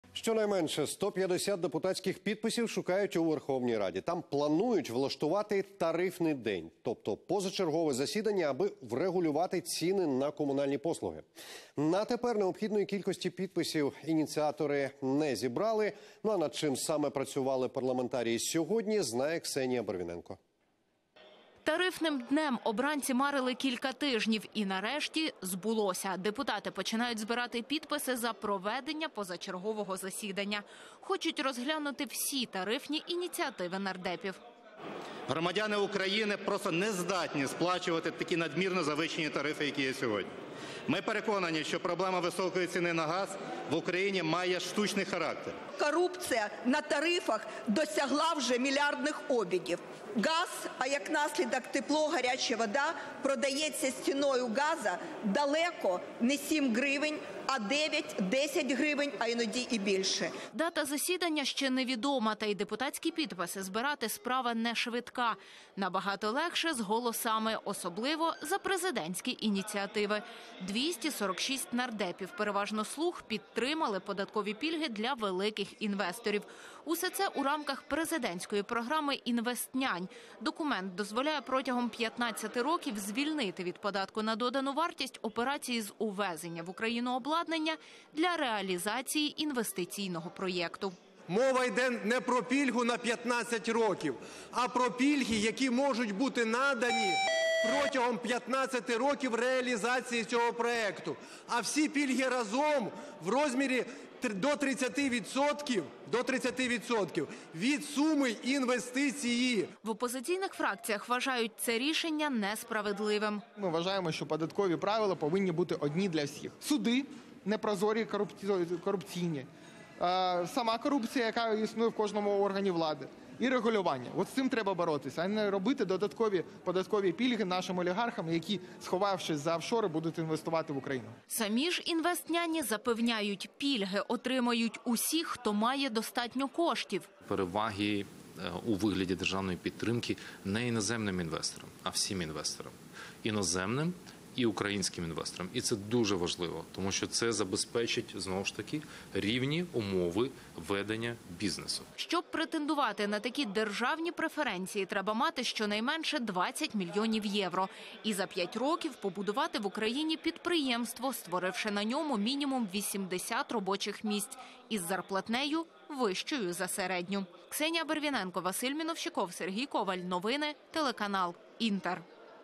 "Тарифний день": у Верховній Раді обговорили космічні борги населення за ЖКП
"Тарифним днем" обранці марили кілька тижнів. І нарешті - збулося, депутати хочуть розглянути всі тарифні ініціативи